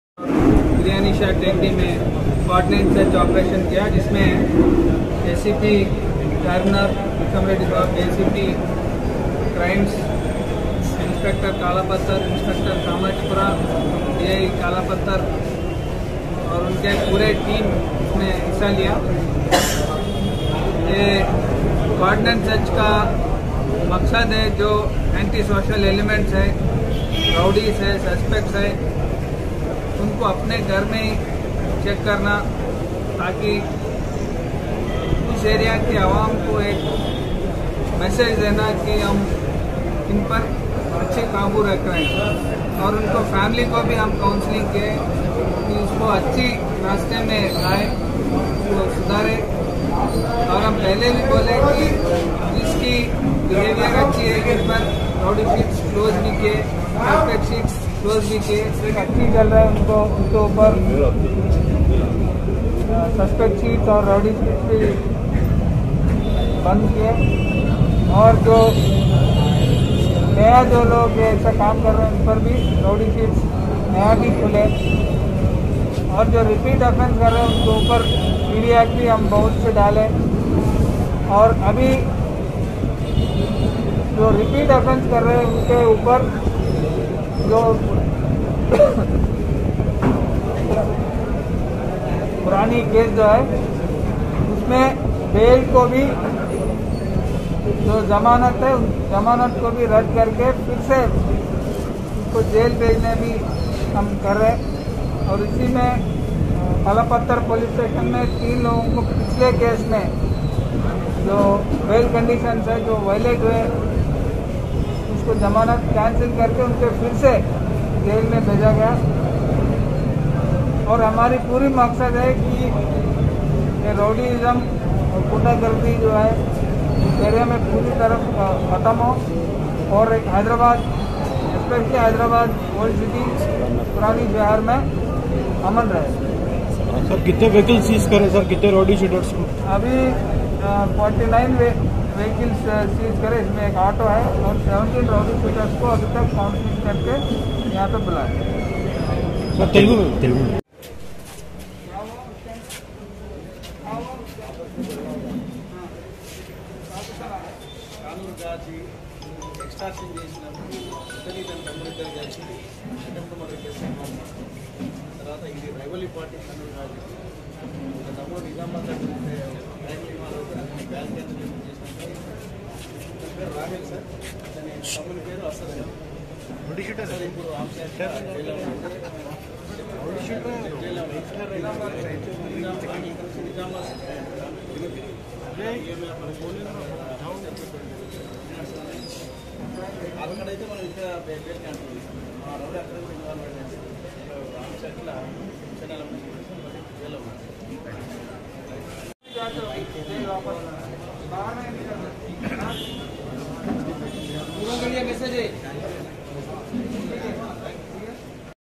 2 SAAL BAAD KIYA GAYA OLD CITY MEIN CORDON & SEARCH | KALAPATHER PS LIMITS | DCP SZ GAJARAO BHOPAL.
2 SAAL BAAD KIYA GAYA PURANE SHAHER KE ILAQE KALAPATHER POLICE HUDOOD MEIN CORDON & SEARCH KARKE 79 MOTORCYCLE'S AUR 1 AUTO KO ZABT KIYA GAYA ISI DAURAN 17 ROWDYSHEETERS KI COUNSELING BHI KI GAYI ! DCP SOUTH ZONE GAJARAO BHOPAL NE BATAYI TAFSEELAT..

FOR DETAILS 👇:-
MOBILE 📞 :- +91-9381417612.

REGARDS
MOHAMMED SOHAIL